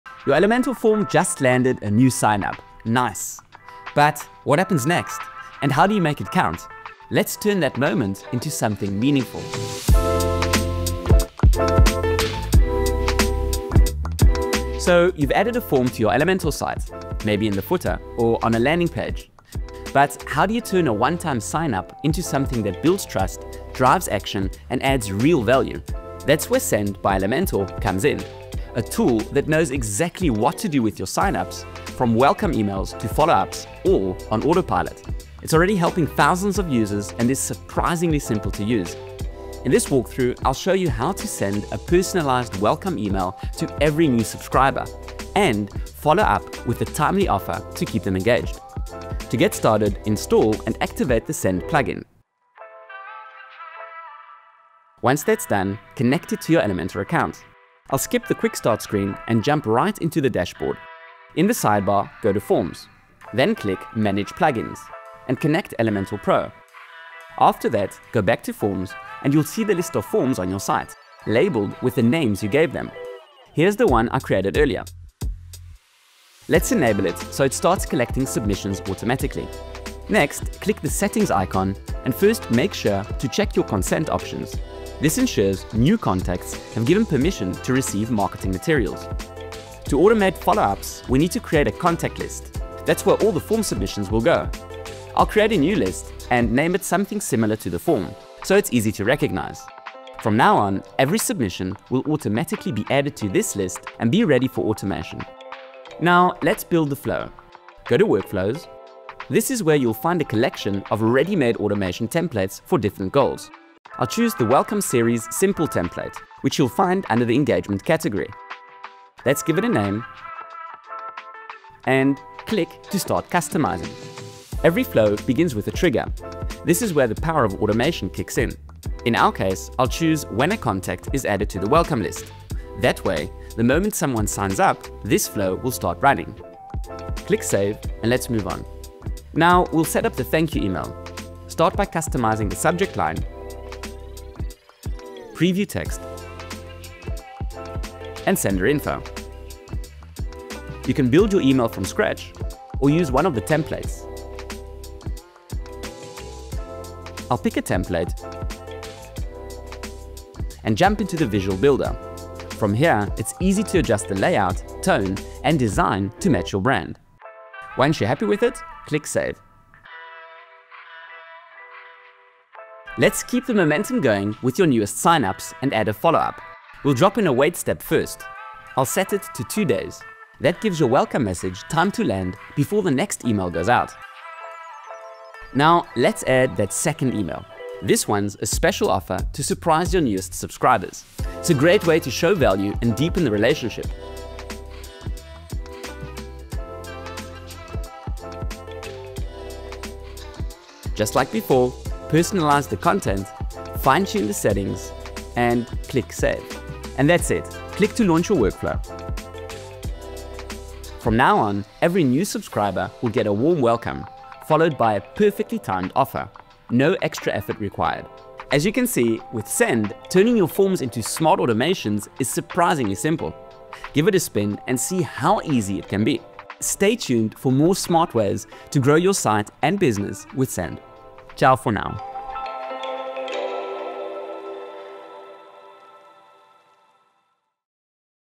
How to Turn Elementor Forms Into Automated Workflows with Send 🚀
📝 Just added a form to your Elementor site?
Great start — now let’s make every signup count.

👉 Meet Send by Elementor: the all-in-one marketing plugin that helps web creators like you turn visitors into loyal customers with simple, effective automation that runs in the background.

In this quick 3-minute walkthrough, you’ll learn how to connect your Elementor forms to Send and build a seamless welcome flow.
No extra tools. Surprisingly simple.

🎯 What you’ll learn:
• How to link Elementor Forms to Send
• How to follow up with personalized offers — all with a visual, drag-and-drop editor

It’s time to turn every form submission into a real opportunity.